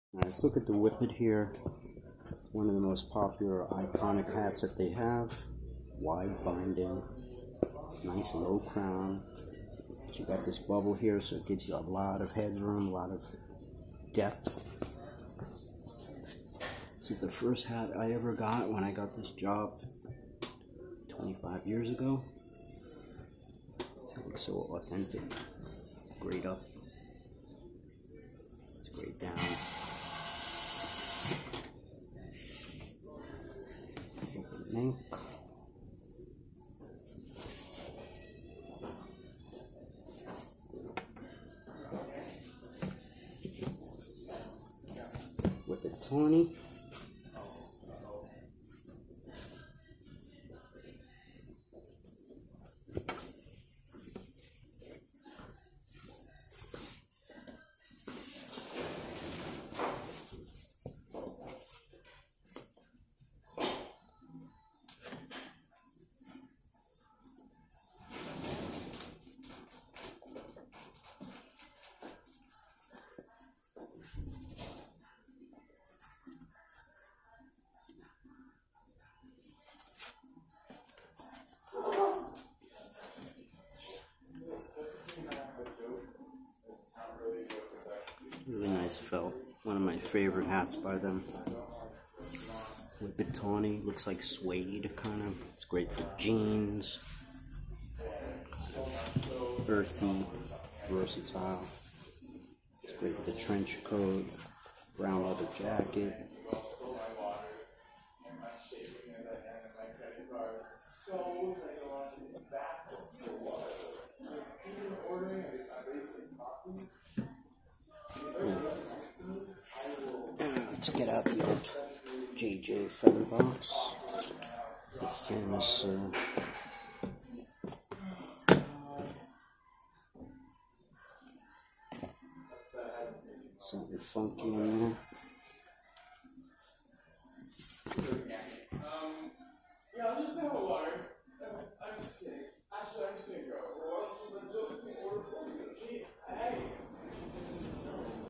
STETSON WHIPPET, My Favorite Stetson Fedora
Sincerely,
An old favorite since a wayyy back when..the iconic STETSON WHIPPET. Maybe my favorite Stetson fedora, and the first ya I bought in 1994 when I took this job.